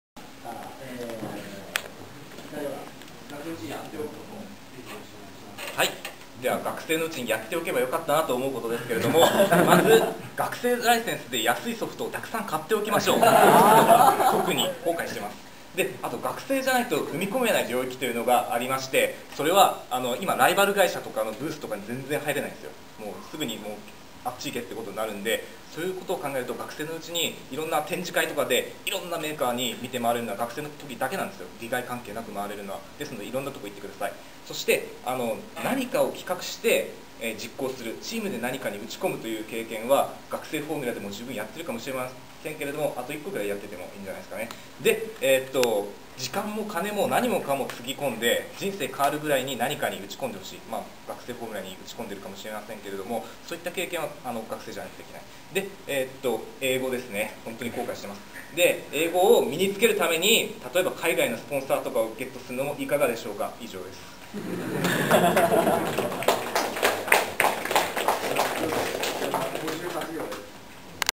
一分間スピーチ 全日本学生フォーミュラ中部支部の勉強会
お題目は確か、学生の頃にやっておけばよかったなーってことを１分で喋るという課題